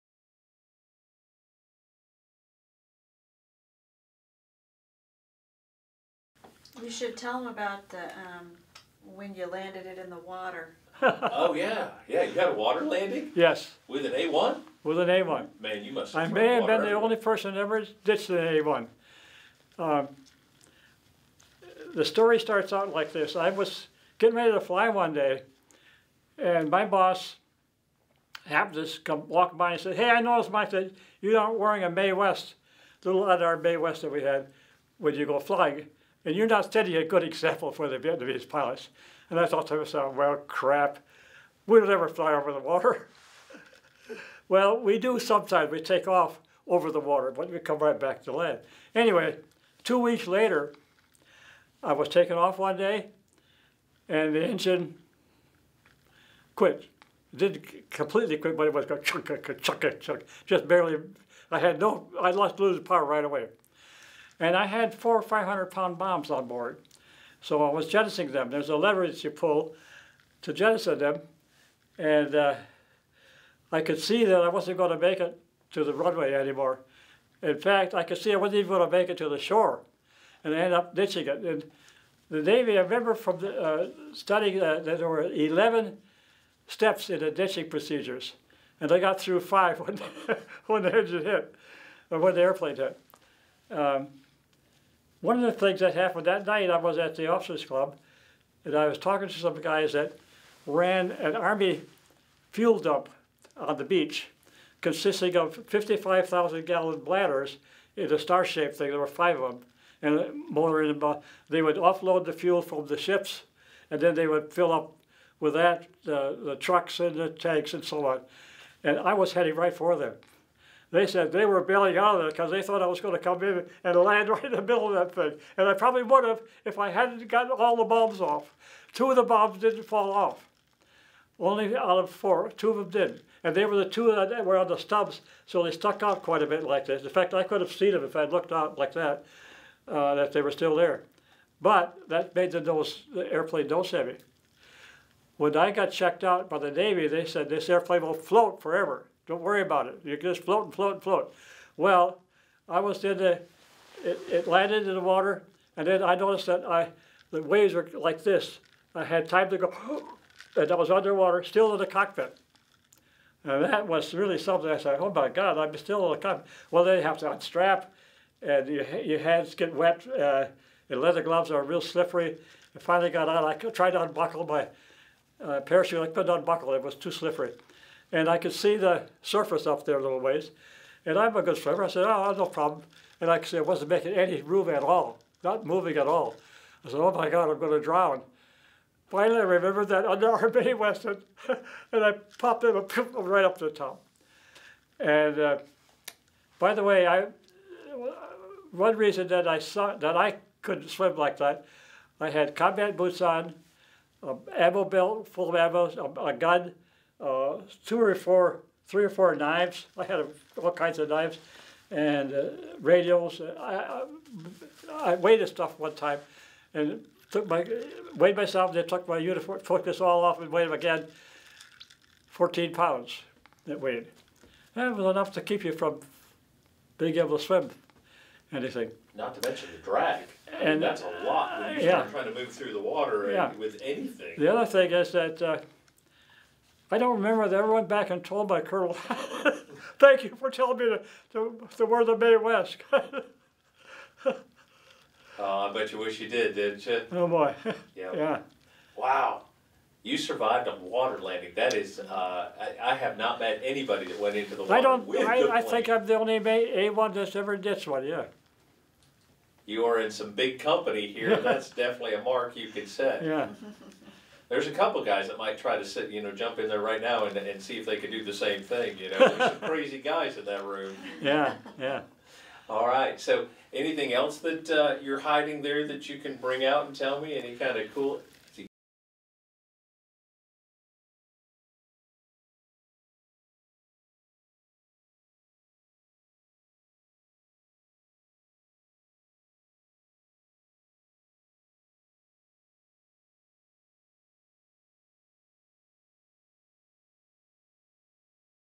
Mike Steger Water Landing in an A1
Mike Steger of the 520th VNAF tell us about the time he did a water landing in an A-1